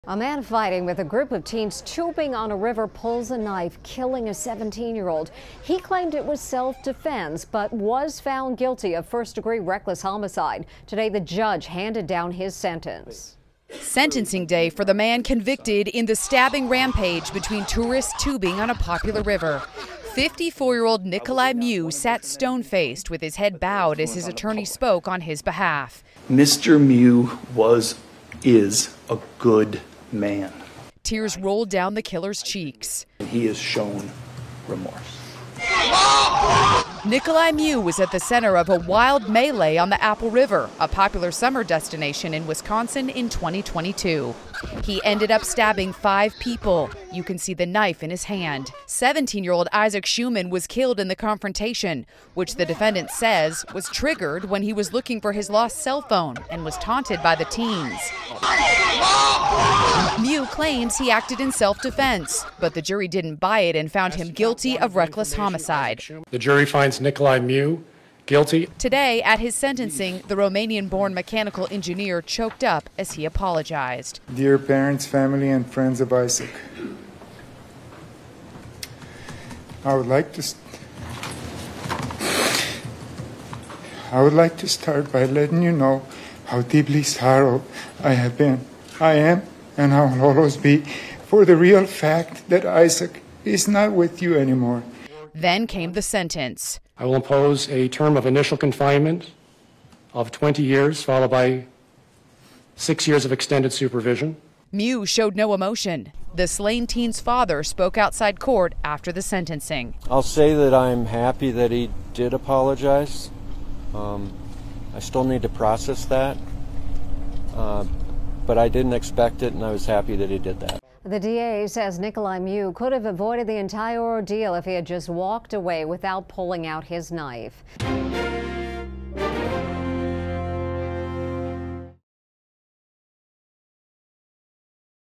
Nicolae Miu Sentenced to 20 Years for Apple River Stabbings
A 54-year-old Minnesota man was sentenced to 20 years in the stabbing attack on tourists tubing on a popular river. Nicolae Miu ended up stabbing five people. Isaac Schuman was killed in the confrontation, which the defendant says was triggered when he was looking for his lost cellphone and was taunted by the teens. Miu claims he acted in self-defense but the jury found him guilty of reckless homicide. At his sentencing, Miu choked up as he apologized.